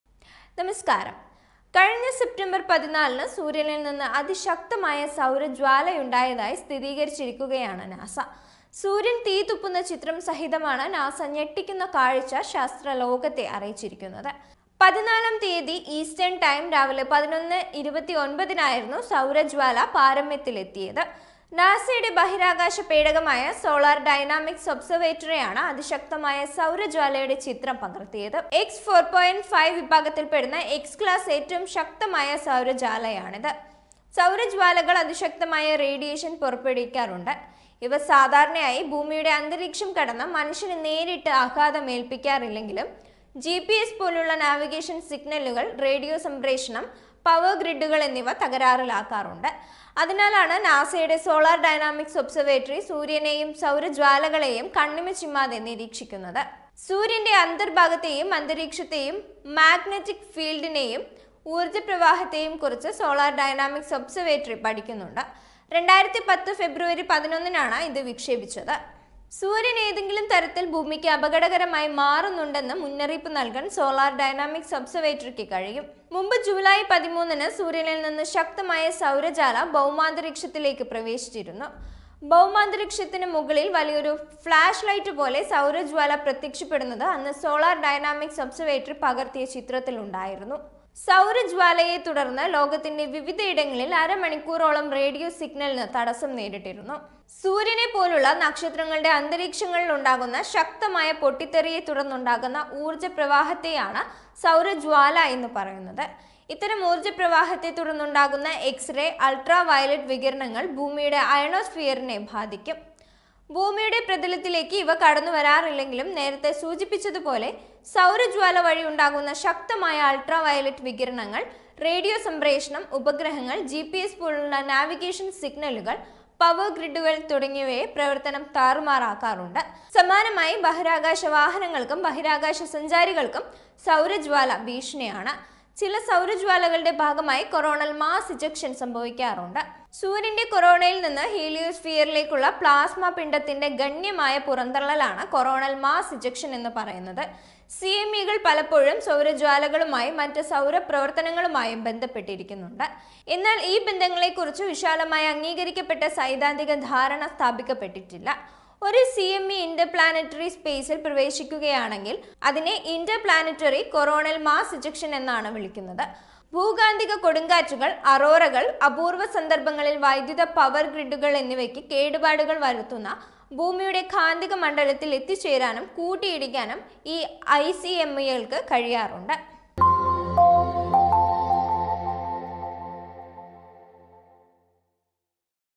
പൊട്ടിത്തെറിച്ച സൂര്യൻറെ ആകാശ വിസ്‌മയം പകർത്തി നാസ.. |the sun emitted a strong solar flare
ഇതെന്താ ഫയറാ..!
കണ്ണടച്ച് തുറക്കും വേഗം
കണ്ണടപ്പിക്കുന്ന തീജ്വാല..!
സൂര്യൻറെ ആകാശ വിസ്‌മയം..